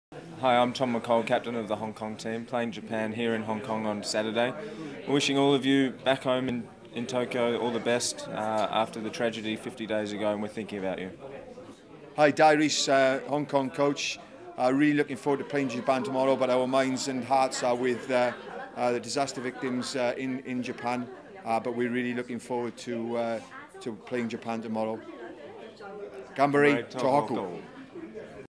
Team Hong Kong supports Tohoku!
Tohoku Gambare! Don't Give up Tohoku! On 30th April it will be exactly 50 days since the Earthquake & Tsunami occurred. A group of Japanese rugby fans have started a campaign to raise money and spirits for people in the affected area with the Tohoku Gambare! Campaign. Organizers hope that this will be a worldwide show of solidarity for the rugby communities of Japan affected by the earthquake.

One of the ways to show support is by voicing a message to the people of Japan affected by this terrible rugby tragedy. Simply explain who you or your team are and where you are playing - and then give this special Japanese message "Gambare Tohoku!" or Don't Give Up Tohoku!

The Hong Kong and Japan teams for the HSBC Asian 5 Nations games and women's curtain raiser did just that today in Hong Kong.  Visit spiritofrugby.com to learn more.